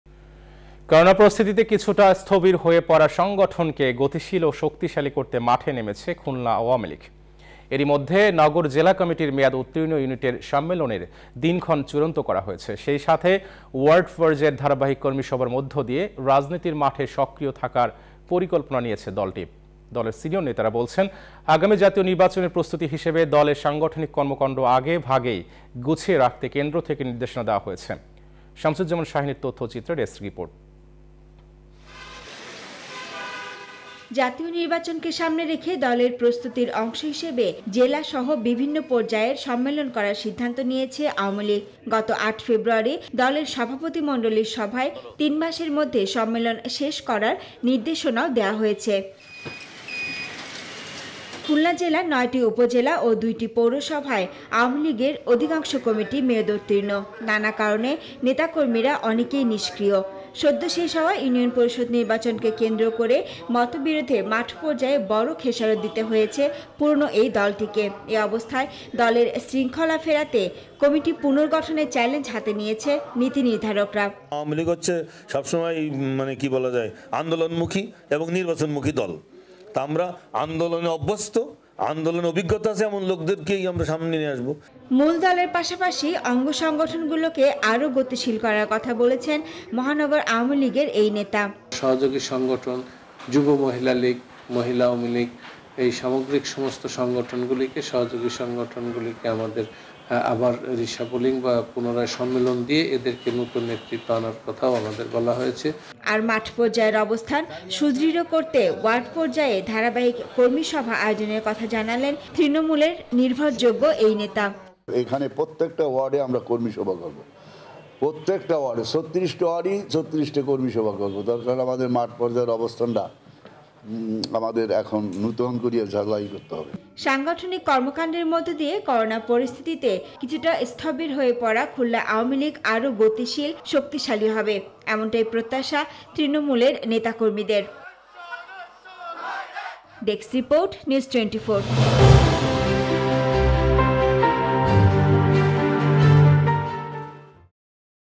নির্বাচন সামনে রেখে তৃণমূলকে ঢেলে সাজাচ্ছে খুলনা আওয়ামী লীগ 27Feb.22| Khulna Awami League
করোনা পরিস্থিতিতে কিছুটা স্থবির হয়ে পড়া সংগঠনকে গতিশীল ও শক্তিশালী করতে মাঠে নেমেছে খুলনা আওয়ামী লীগ। এরই মধ্যে নগর ও জেলা কমিটির মেয়াদ উত্তীর্ণ ইউনিটের সম্মেলনের দিনক্ষণ চূড়ান্ত করা হয়েছে। সেইসাথে ওয়ার্ড পর্যায়ে ধারাবাহিক কর্মীসভার মধ্যে দিয়ে রাজনীতির মাঠে সক্রিয় থাকার পরিকল্পনা নিয়েছে দলটি। দলের সিনিয়র নেতারা বলছেন, আগামী জাতীয় নির্বাচনের প্রস্তুতি হিসেবে দলে সাংগঠনিক কর্মকান্ড আগে ভাগেই গুছিয়ে রাখতে কেন্দ্র থেকে নির্দেশনা দেয়া হয়েছে। সামছুজ্জামান শাহীনের তথ্য ও চিত্রে ডেস্ক রির্পোট ।

জাতীয় নির্বাচনকে সামনে রেখে দলের প্রস্তুতির অংশ হিসেবে জেলাসহ বিভিন্ন পর্যায়ের সম্মেলন করার সিদ্ধান্ত নিয়েছে আওয়ামী লীগ। গত ৮ ফেব্রুয়ারি দলের সভাপতিমন্ডলীর সভায় তিন মাসের মধ্যে সম্মেলন শেষ করার নির্দেশনাও দেয়া হয়েছে।

খুলনা জেলার নয়টি উপজেলা ও দুইটি পৌরসভায় আওয়ামী লীগের অধিকাংশ কমিটির মেয়াদ উত্তীর্ণ। নানা কারণে নেতাকর্মীরা অনেকেই নিষ্ক্রিয়। সদ্যশেষ হওয়া ইউনিয়ন পরিষদ নির্বাচনকে কেন্দ্র করে মতদ্বন্দ্বে মাঠ পর্যায়ে বড় খেসারত দিয়েছে পুরোনো এই দলটি। বিভিন্ন ইউনিয়নে নৌকার বিদ্রোহী প্রার্থী ও তাদের সমর্থনে সক্রিয় থাকায় দুই শতাধিক নেতাকর্মীকে বহিস্কার করা হয়েছে। এ অবস্থায় দলের শৃঙ্খলা ফেরাতে কমিটি পুনর্গঠনের চ্যালেঞ্জ হাতে নিয়েছে নীতি-নির্ধারকরা।

এদিকে মূল দলের পাশাপাশি অঙ্গসংগঠনগুলোকে আরও গতিশীল করার কথা বলেছেন মহানগর আওয়ামী লীগের এই নেতা

আর মাঠ পর্যায়ের অবস্থান সুদৃঢ় করতে ওয়ার্ড পর্যায়ে ধারাবাহিক কর্মীসভা আয়োজনের কথা জানালেন তৃনমূলের নির্ভরযোগ্য এই নেতা

সাংগঠনিক কর্মকান্ডের মধ্যে দিয়ে করোনা পরিস্থিতিতে কিছুটা স্থবির হয়ে পড়া খুলনা আওয়ামী লীগ আরও গতিশীল- শক্তিশালী হবে এমনটাই প্রত্যাশা তৃনমূলের নেতাকর্মীদের।

Our other YouTube channels :
News24 Official Address:
NEWS24
371/A
Bashundhara Road
Block – D
Bashundhara Residential Area
Dhaka - 1229